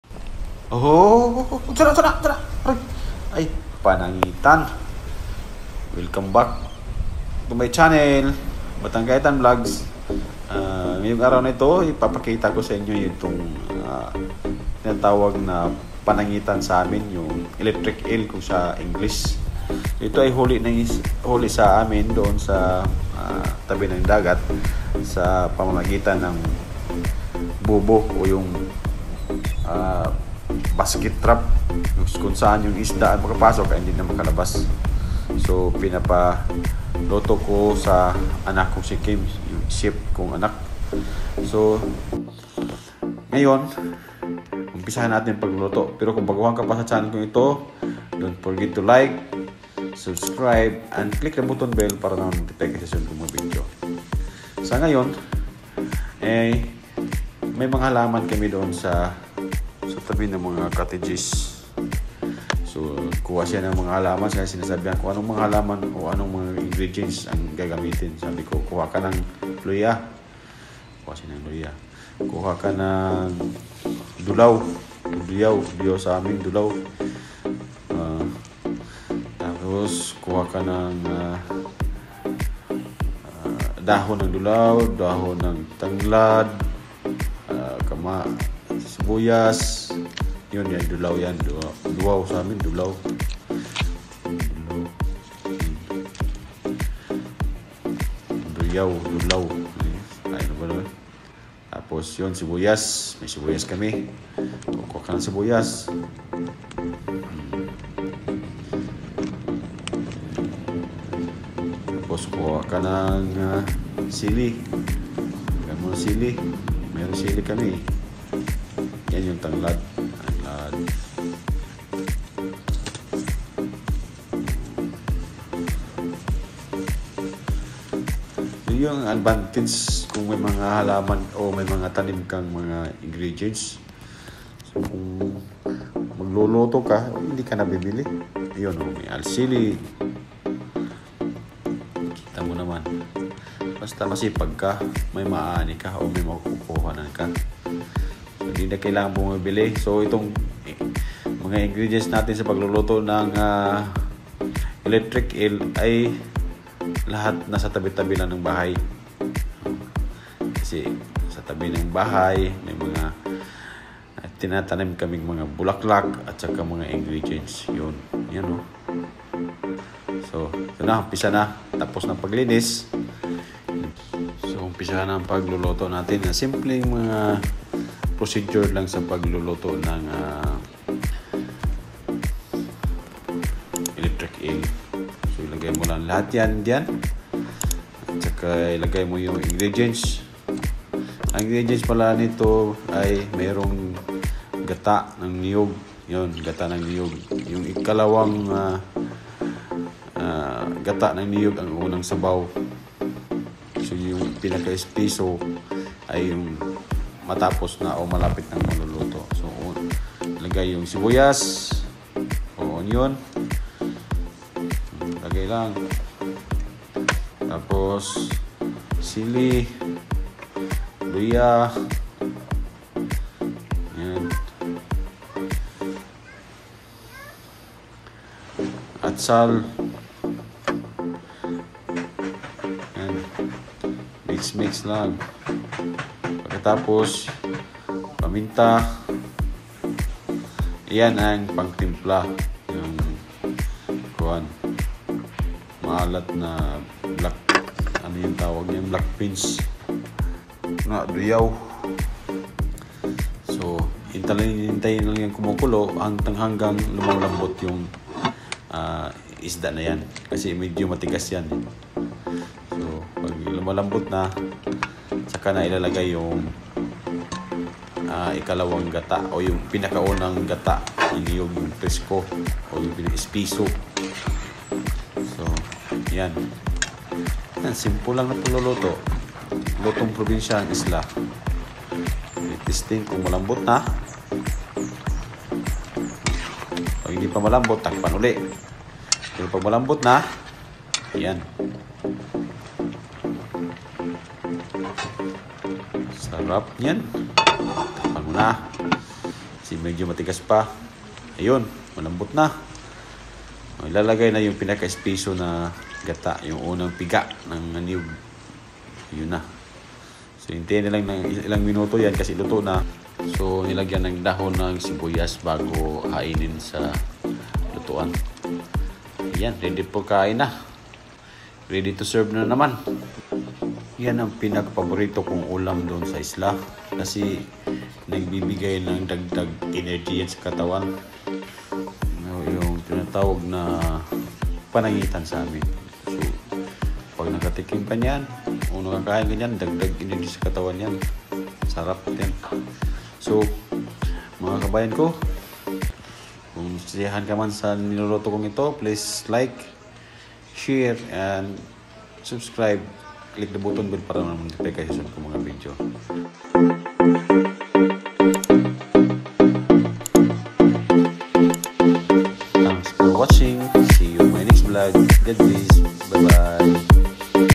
Pangdagdag energy’s sa katawan, Ginataang Panangitan. Eel fish recipe
One  of MY FAVORITE EEL FISH RECIPE in the Island “Panangitan in bisaya”.
Adding energy to my body base on my experience.
If your are first timer to taste the eel fish/recipe you will amized and you start looking for this eel fish/recipe again.